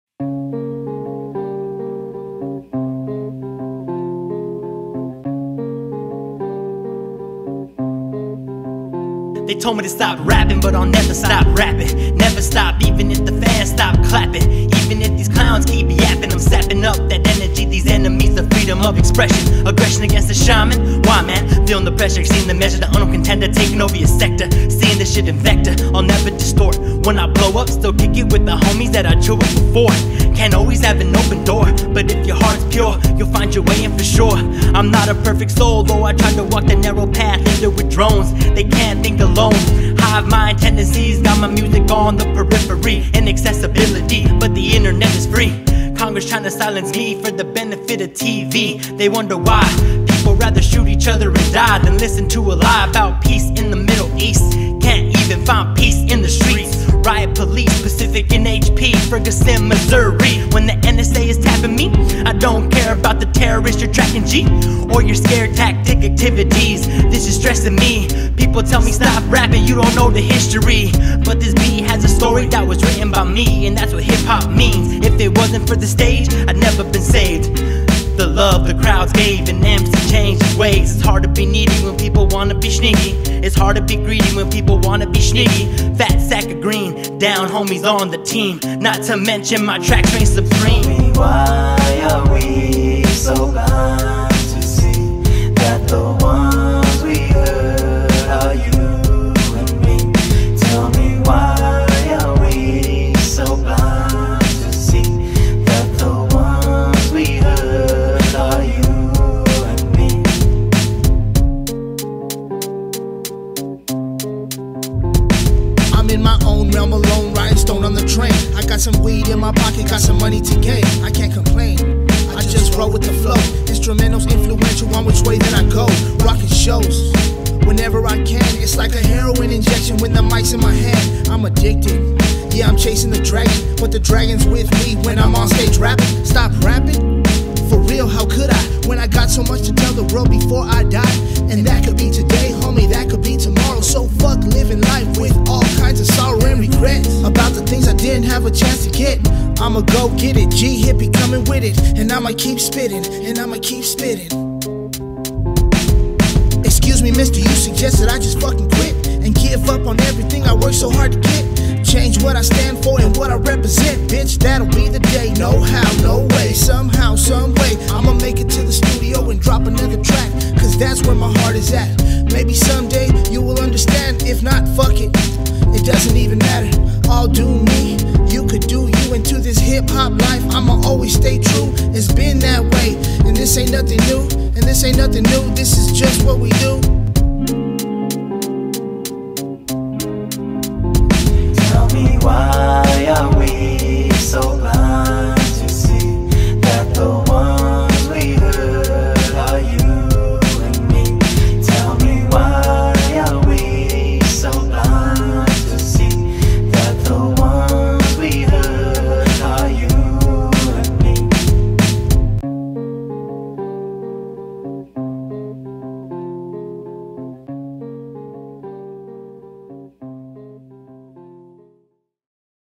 Birthday Celebration at the Redwood Room
SHNEAKY'S B-Day Celebration at Speak Easy Maywood hosted by Cali Ally inside the historical Redwood Room. Performances by Echo, Kasper, FACE and Barlos, Demise, D'range, Destro Suave and Kchris. Shot and Edited by Napalm Monkey Productions